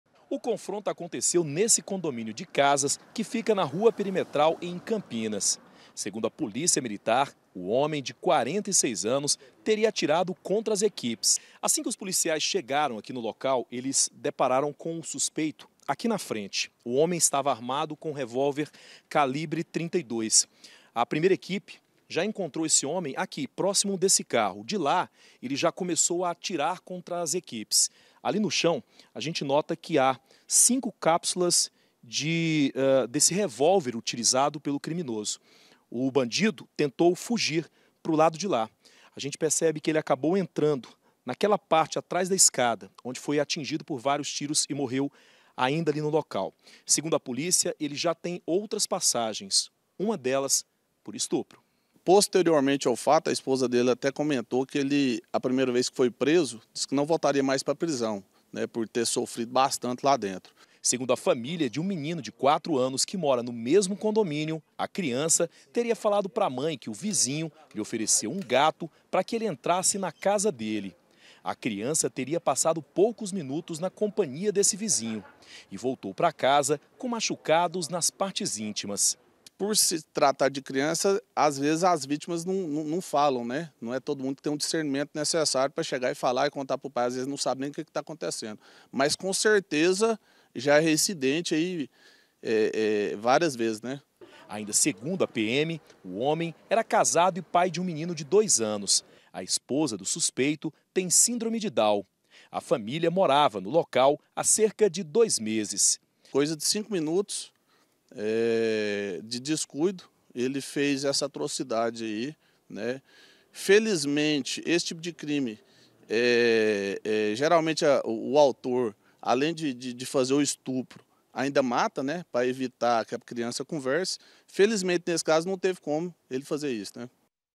CONFRONTO COM A PM: SUSPEITO DE ABUSO CONTRA CRIANÇA MORRE EM TIROTEIO
UM HOMEM DE 46 ANOS MORREU EM CONFRONTO COM A POLÍCIA MILITAR, NUM CONDOMÍNIO DE CASAS EM CAMPINAS. OS POLICIAIS FORAM CHAMADOS PORQUE O HOMEM TERIA ABUSADO DE UM MENINO DE 4 ANOS, QUE MORA NO MESMO CONDOMÍNIO. E QUANDO CHEGARAM, TERIAM SIDO RECEBIDOS A BALA.